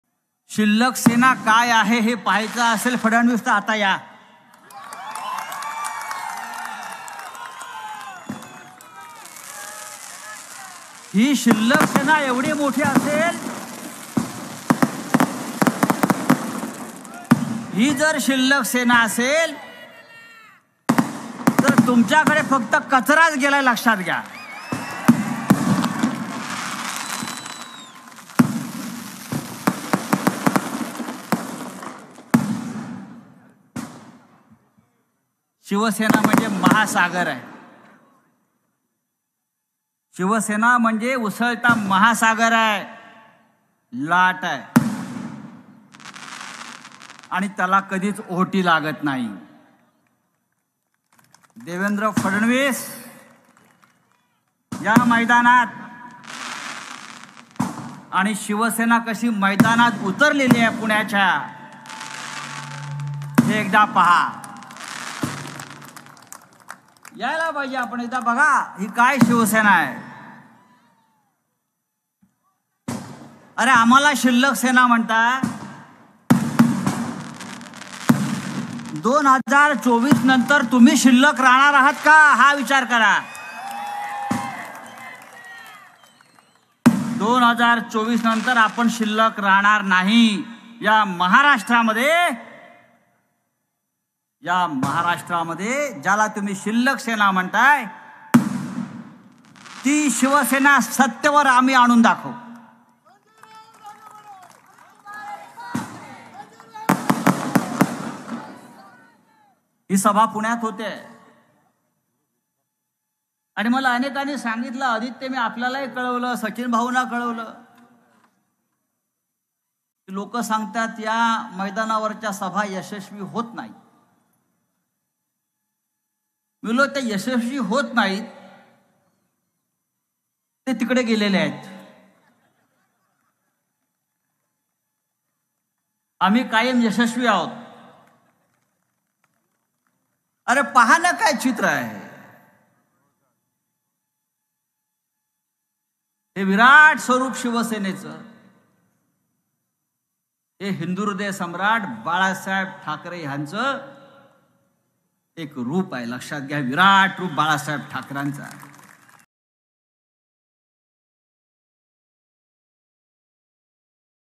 राऊतांनी भर सभेत फडणवीसांना ठणकावले, जबरदस्त बोलले, दिलं आव्हान | Sanjay Raut angry on Fadnavis |RA4
राऊतांनी भर सभेत फडणवीसांना ठणकावले, जबरदस्त बोलले, दिलं आव्हान | Sanjay Raut angry on Fadnavis